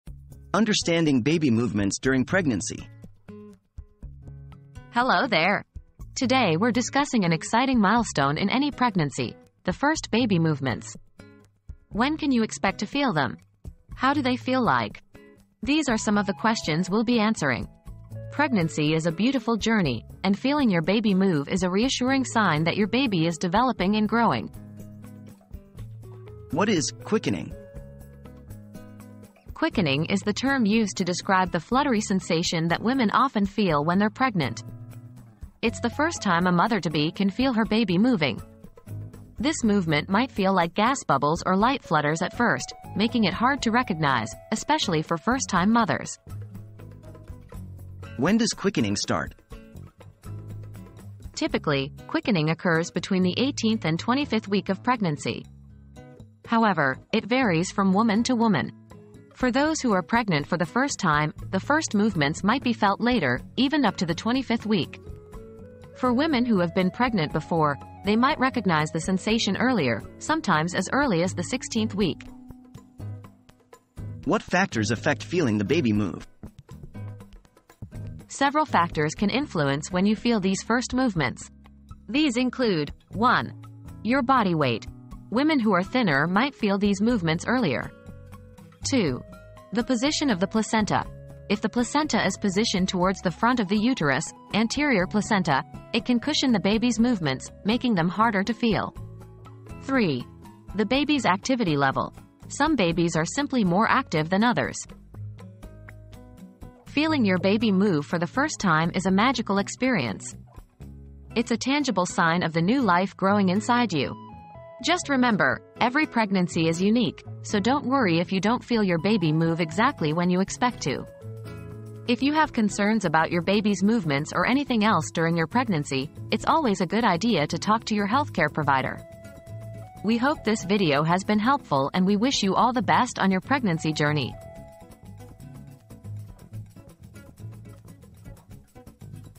Understanding Baby Movements During Pregnancy
When can you first feel baby movement during pregnancy? • Discover the exciting milestone of feeling your baby move during pregnancy! Learn when to expect those magical fluttery sensations and what factors can affect when you feel them. Join us as we explore the beautiful journey of pregnancy and the reassuring signs of your baby's growth and development.

00:00 • Understanding Baby Movements During Pregnancy
00:26 • What is "Quickening"?
00:48 • When Does Quickening Start?
01:14 • What Factors Affect Feeling the Baby Move?